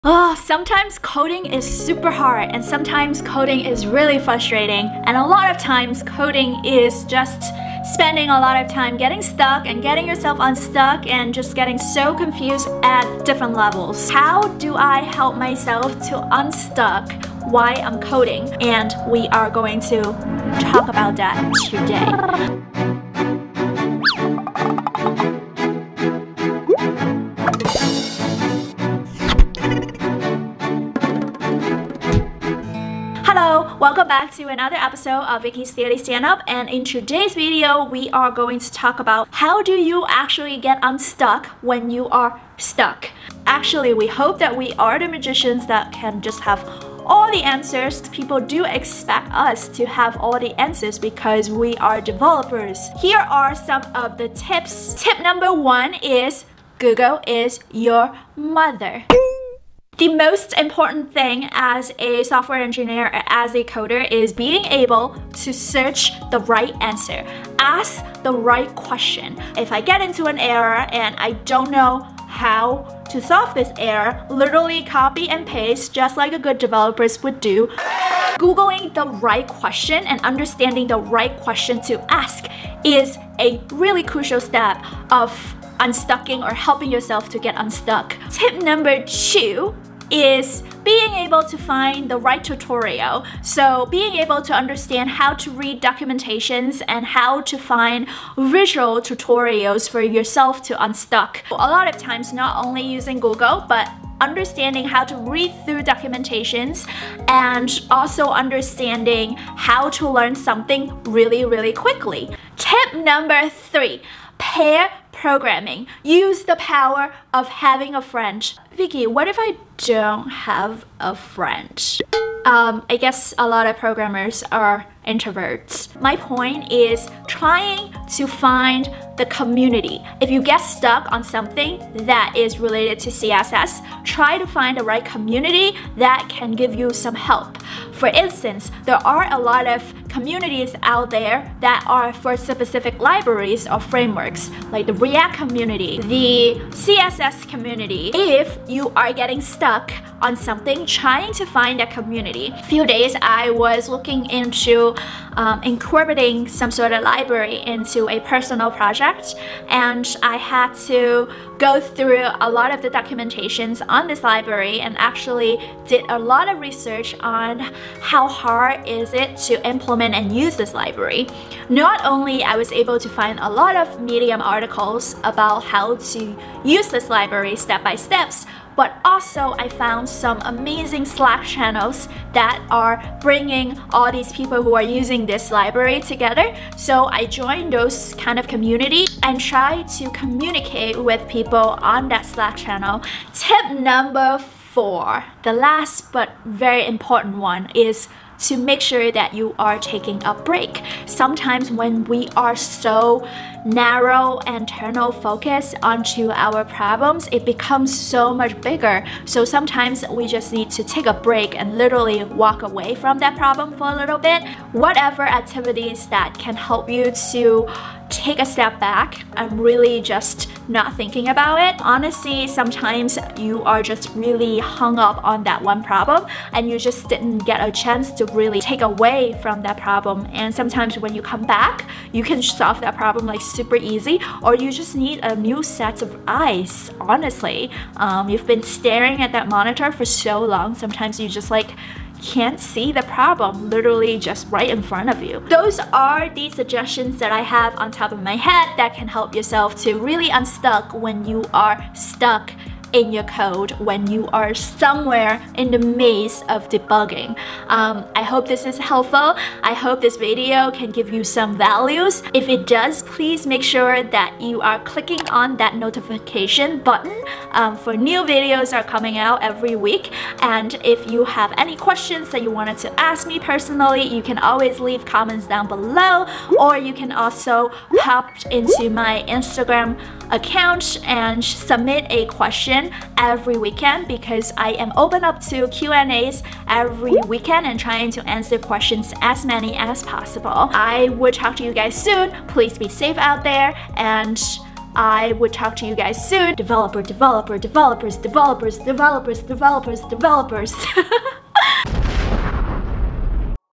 How to get Unstuck While Coding | All Programmers Get Stuck While Learning
How to get Unstuck While Coding? How to get out of being stuck when you learning to code? Oh trust me I have been there! I was stuck all the time, and learn how to help myself out.  In this video, I am going to provide 4 tips and tricks that i learned from the past to help myself unstuck.